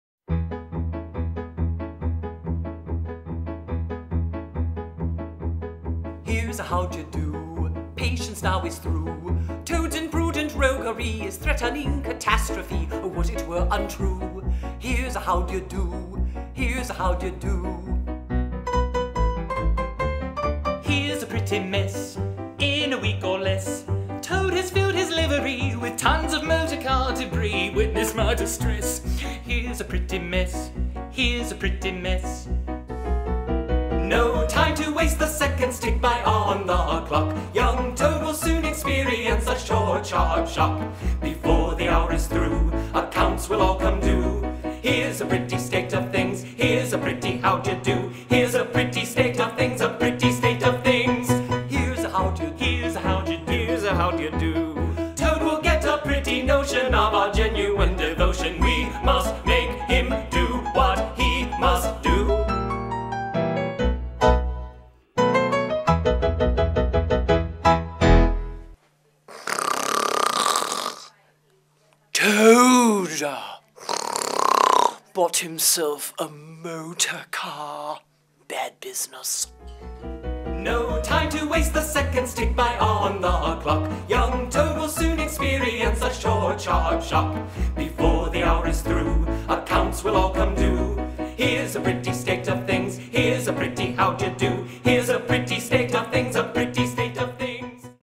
Becoming Badger - NCT's The Wind In The Willows
It takes Rona Carter 45 minutes to become what is a now one of her legendary roles, Badger...but you get to see it in ONE at 4000x speed!

See her at Nashville Children's Theatre in our production of The Wind In The Willows, running until March 24th